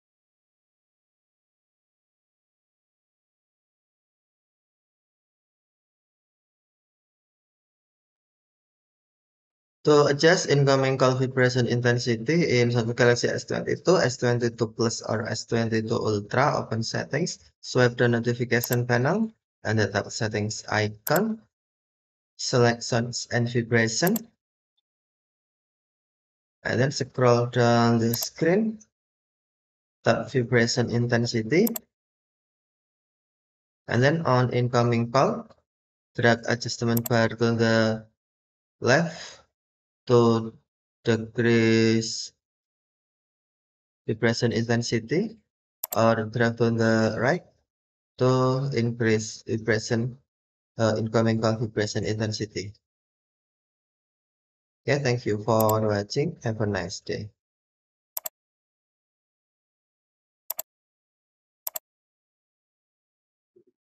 Samsung Galaxy S22 Ultra : How to adjust Incoming call Vibration intensity
In this video, you will find the steps on How to adjust Incoming call Vibration intensity in Samsung Galaxy S22/S22+/S22 Ultra.  This video uses Samsung Galaxy S22 Ultra SM-S908E/DS International version runs with Android 12